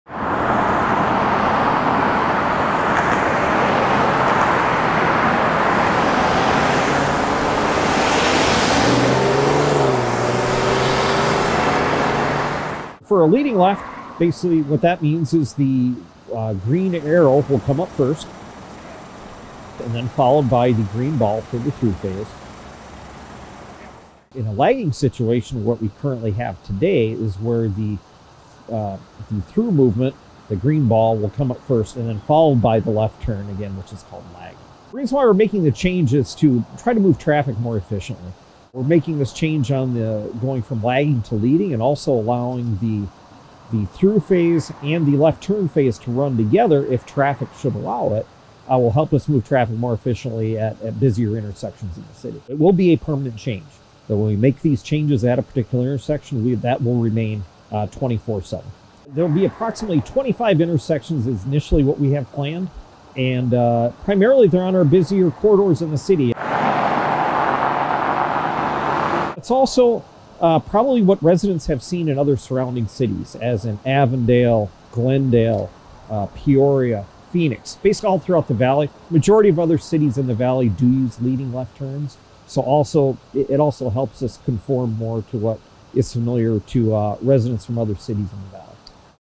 Traffic lights changes coming to Goodyear
Be on the lookout for changes that will be coming to some Goodyear traffic signals as the city transitions to leading left turn arrows.